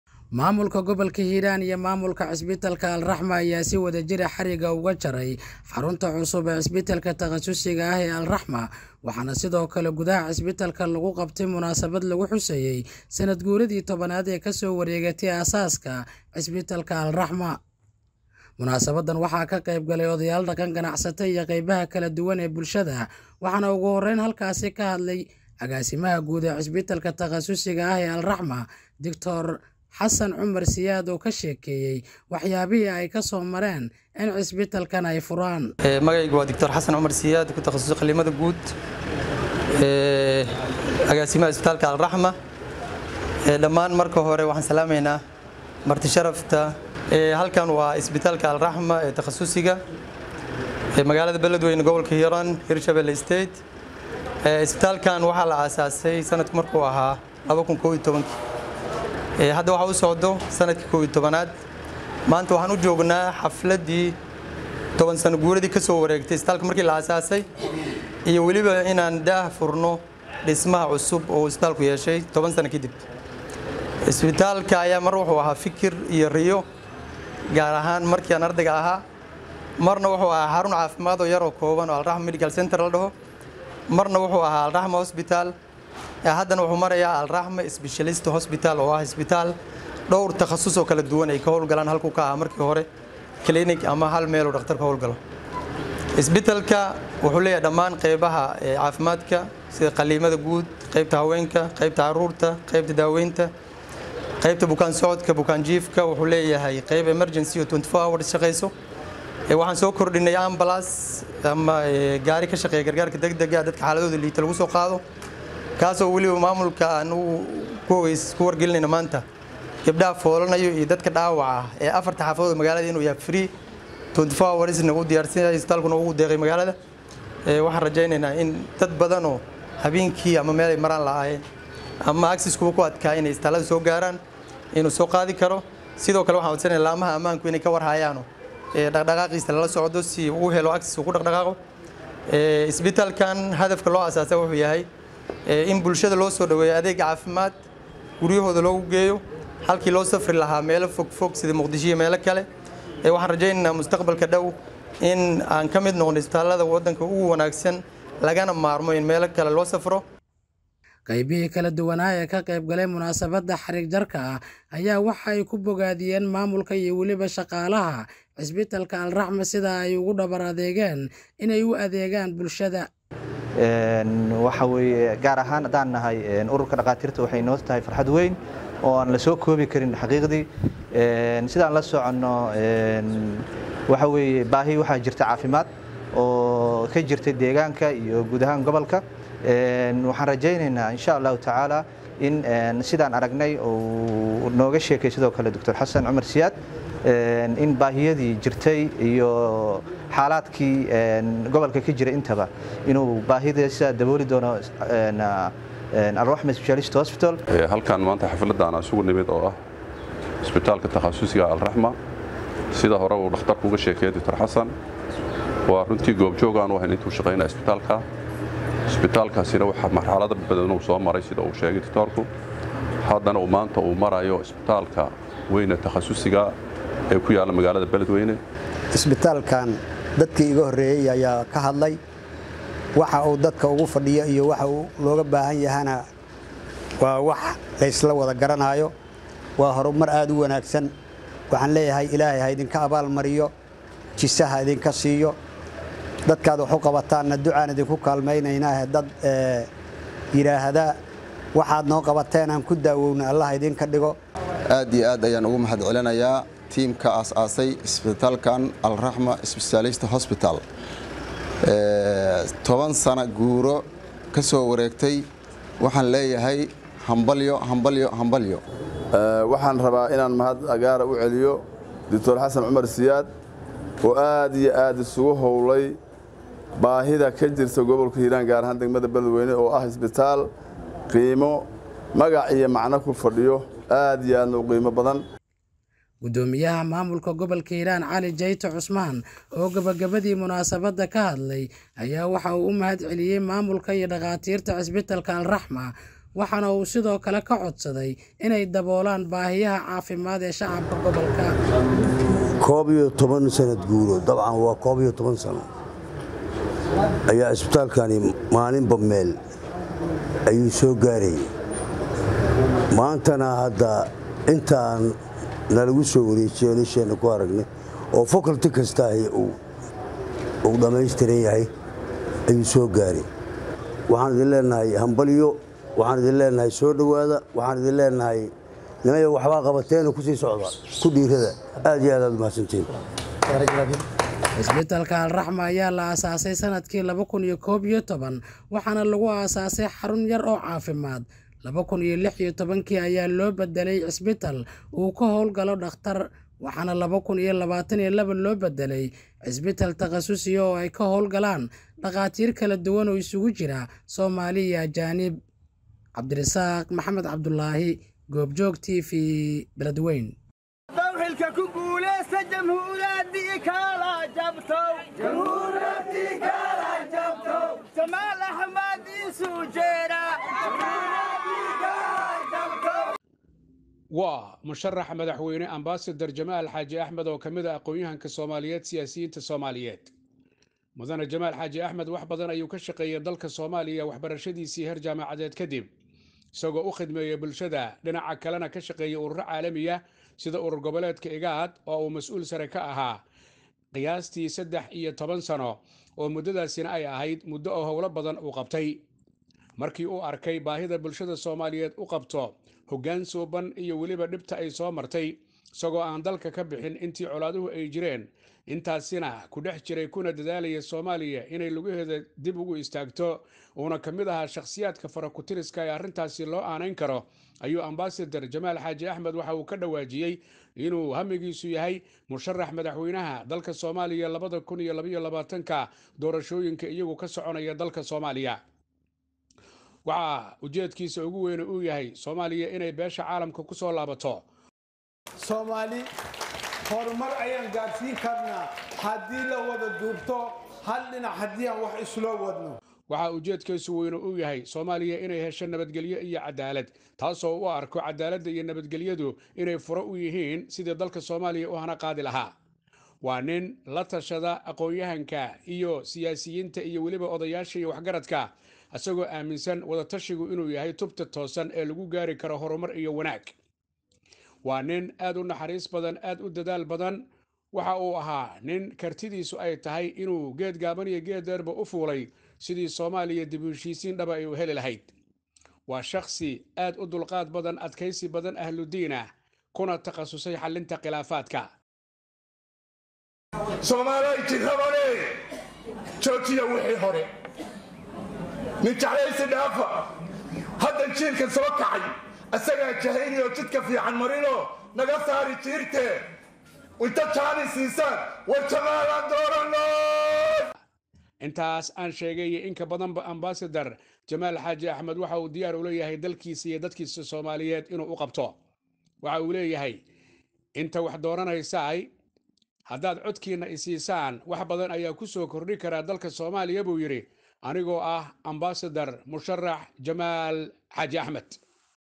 Maamulka Gobalka Hiraan ayaa maanta xariga ka jaray dhismaha cusub ee cisbitaalka
Maamulka Gobalka Hiraan ayaa maanta xariga ka jaray dhismaha cusub ee cisbitaalka Alraxma oo dhaqaatiir kala duwan aay ka howlgali doonaan.